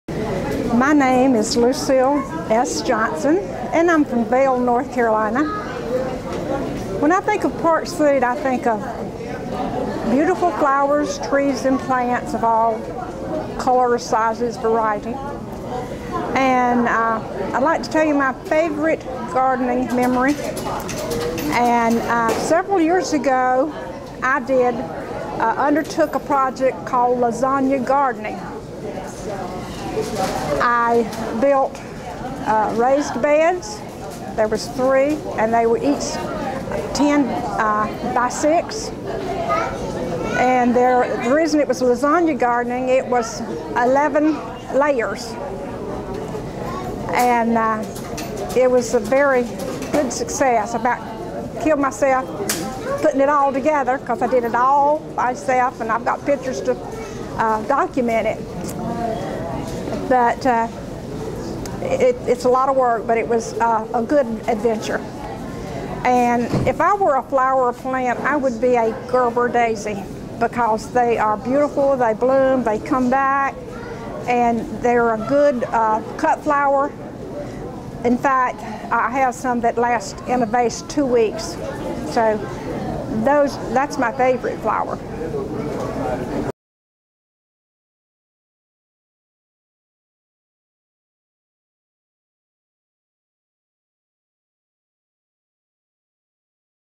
Park Seed Flower Day 2008: Lucille S. Johnson
Lucille Johnson tells about her favorite gardening memory. She tells of a project called Lasagna Gardening.